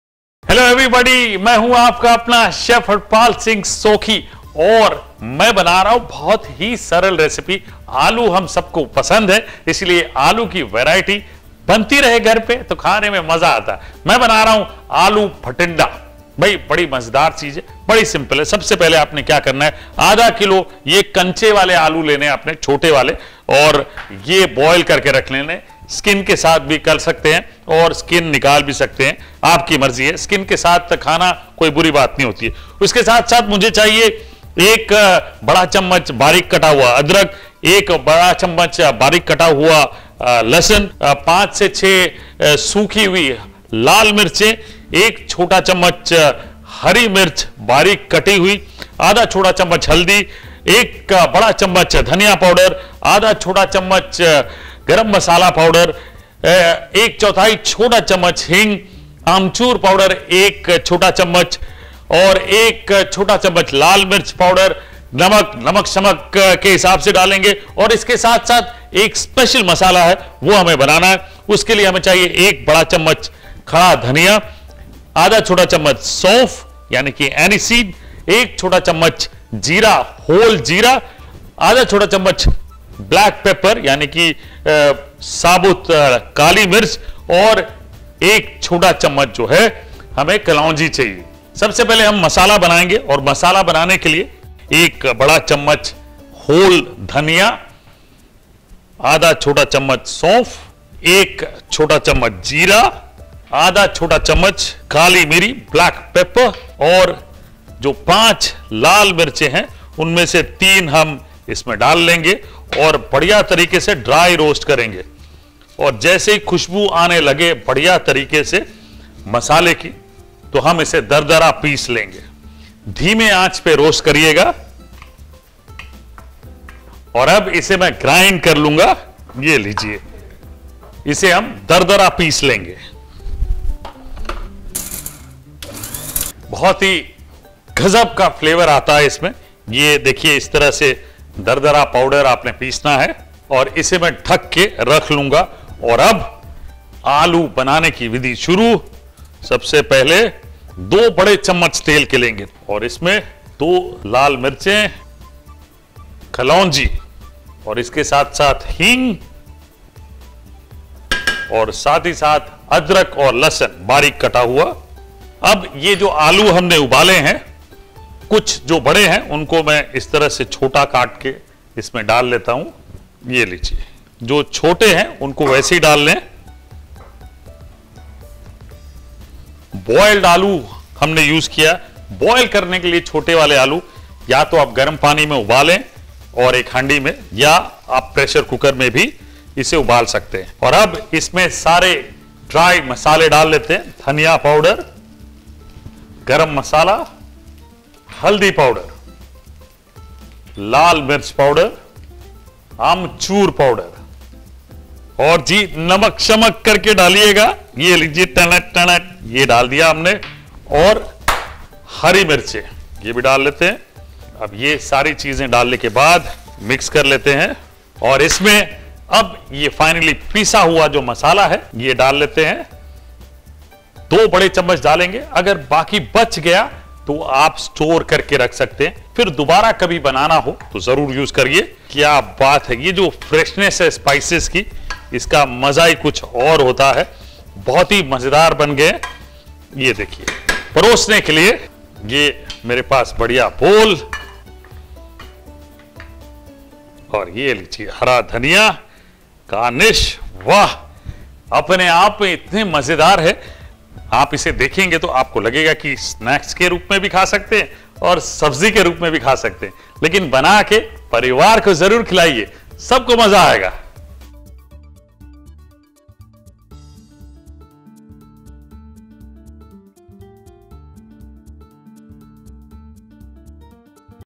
Aloo Bhatinda | Punjabi recipe |Chef Harpal Singh
Baby potatoes are easy to cook and experiment with. Here is a dry Punjabi dum aloo recipe that can be made in minutes.

Ingredients

Baby potatoes boiled and peeled  ½ kg
Spice mixture     2 Tbsp
Dry Red chili    5-6
Green chili chopped    1 tsp

For Spice mixture
Coriander seeds    1 Tbsp
Aniseed     ½ tsp
Cumin seeds     1 tsp
Peppercorn     1/2 tsp
Kalonji onion seeds    2 tsp

Method
• In a pan add coriander seeds, cumin seeds, 3 dry red chili , peppercorn, aniseeds and dry roast till fragrant.
• Add the mixture to a blender and coarsely grind and keep aside.
• In a pan heat oil, add 2 dry red chili, Kalonji, Hing, garlic chopped, ginger chopped, baby potatoes, coriander powder, Garam masala, turmeric powder, Red chili powder, turmeric powder, green chili chopped, salt and the coarsely ground spice mixture 2 tbsp.
• Mix well and remove in a serving bowl
• Garnish with coriander sprig and Serve hot